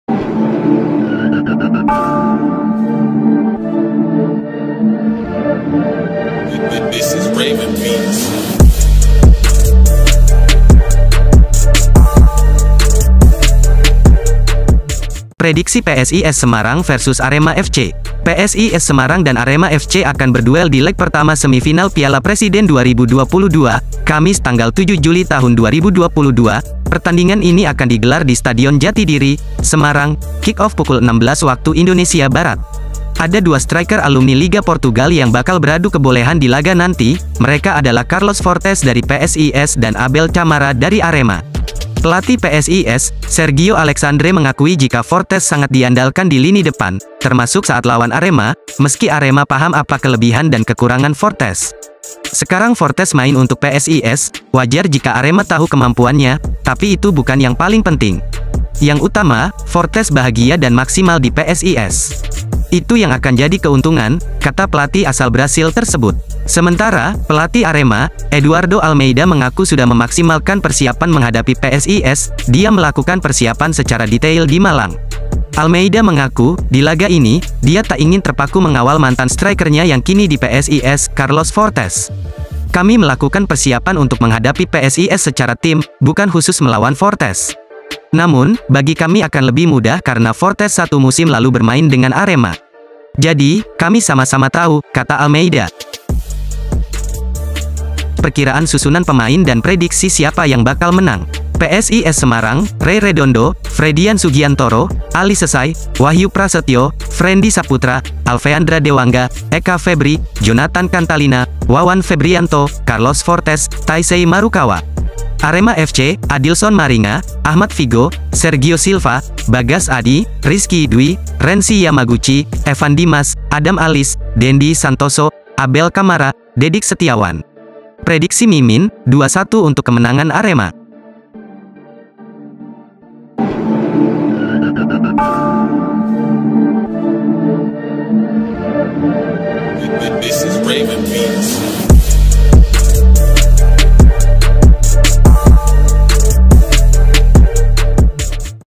OPTIMIS 3 POIN!! Ada Predator Baru!! Inilah Prediksi Susunan Pemain Arema!! PSIS VS Arema
thumbnail: aremadsgn